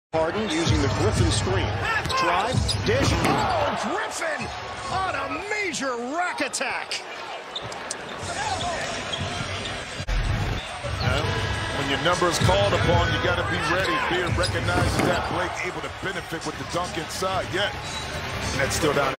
Blake Griffin great dunk Harden great pass vs Suns
guidelines of YouTube .